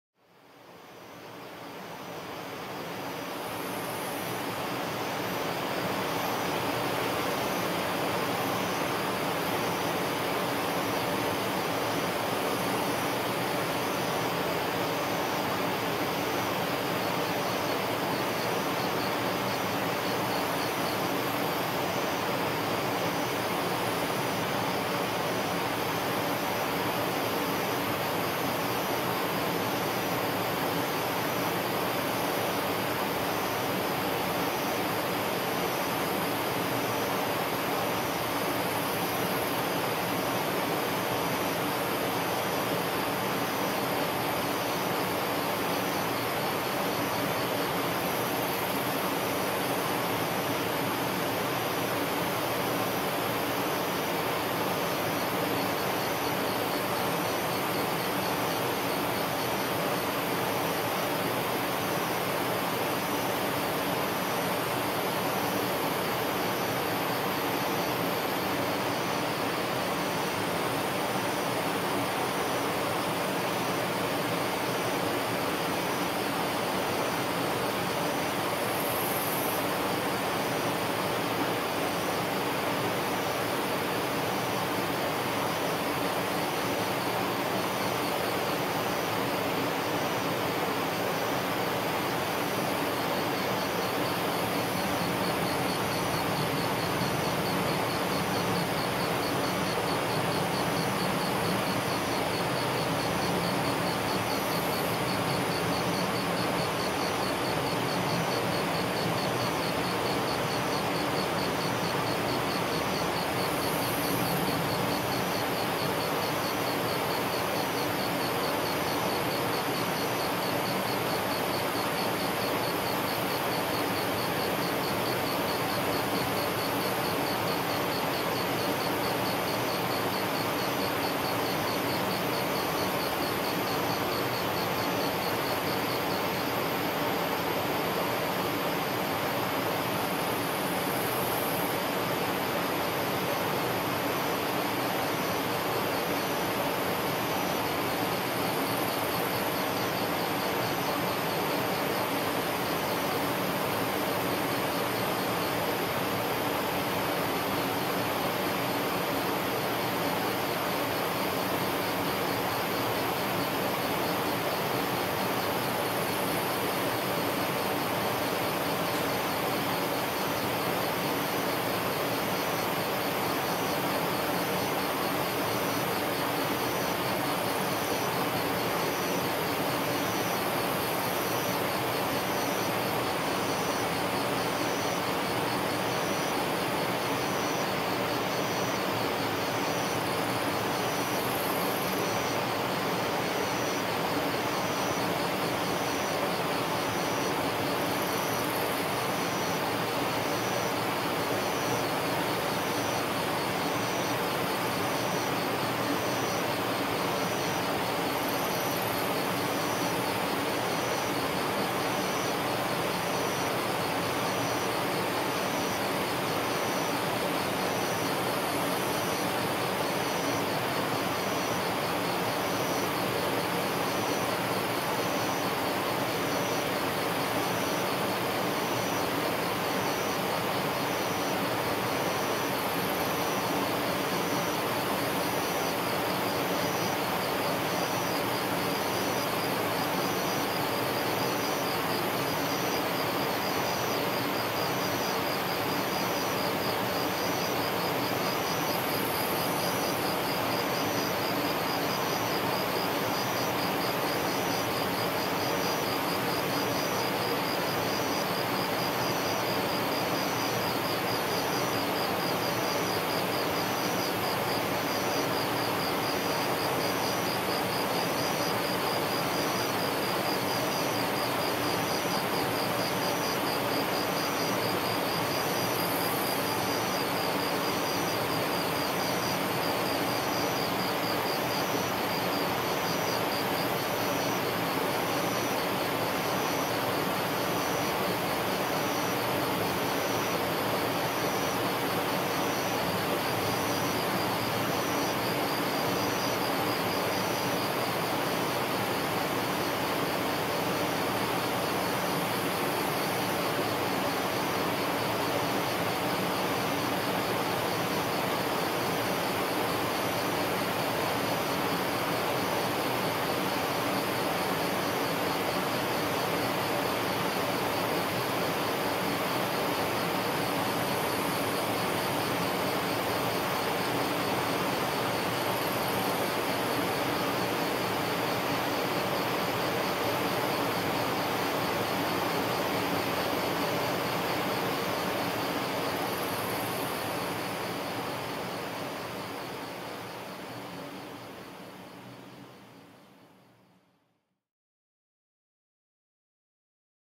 小菅 - 秋の虫01#4【HPL4】
山梨県小菅村 白糸の滝近くで収録した秋の虫の鳴き声です
背後で川の流れる音も入っています
ヘッドフォンまたはイヤフォンの使用をお勧めします

Integrated Loudness：-24.0LKFS
レコーダー：ZOOM H2n (24bit/48kHz 4ch収録)
編集：4chサラウンド音声をNovoNotes 3DXでHPL（バイノーラル）に変換
収録日：2018年8月